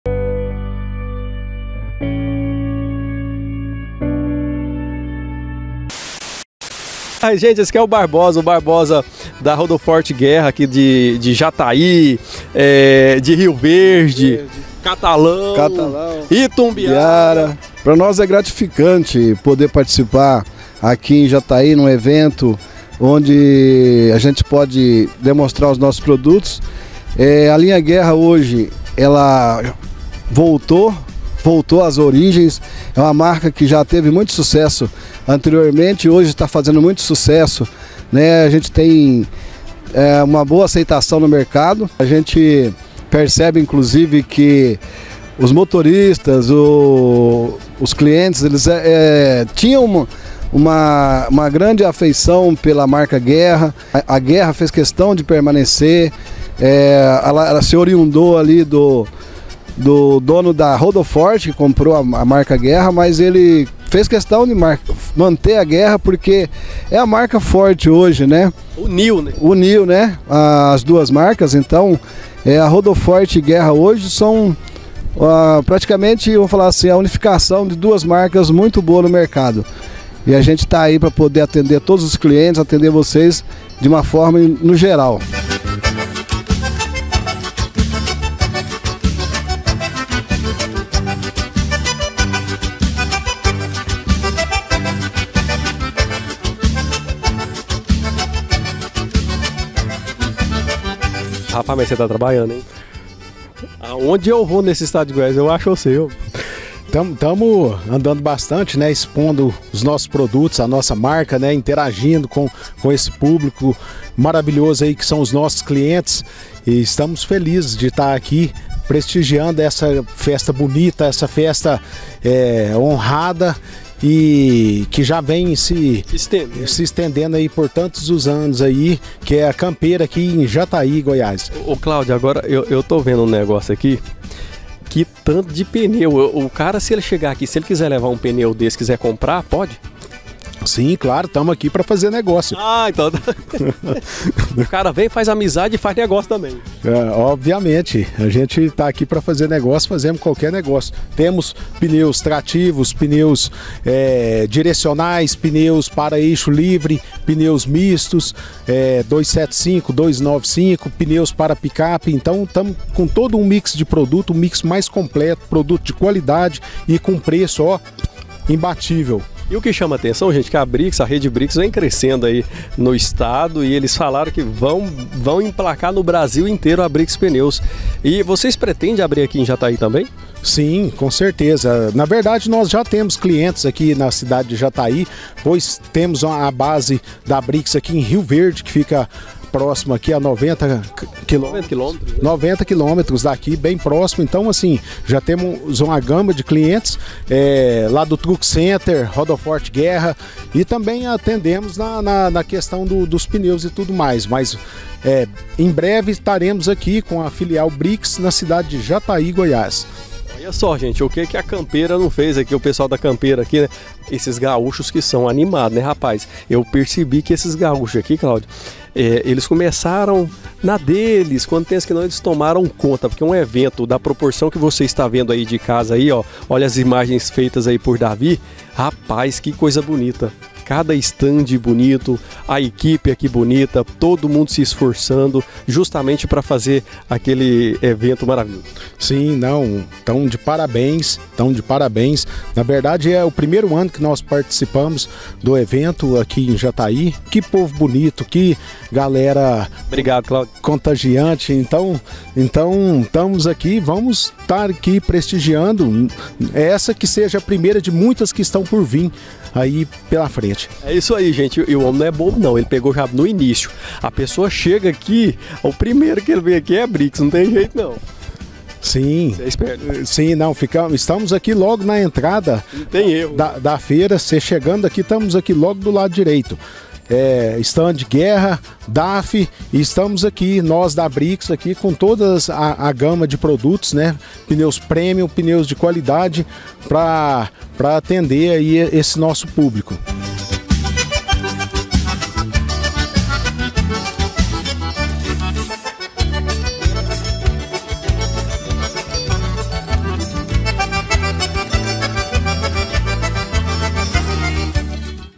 CAMPEIRA 2022 - BRIXX E RODOFORT GUERRA
A BRIXX PNEUS e RODOFORT GUERRA estavam presentes na Campeira Jataí 2022.

Um Evento realizado pelo CTG todo ano na região sudoeste do estado de Goiás.

Conteúdo Produzido pela empresa Mais Mídia Brasil - Amigos da Estrada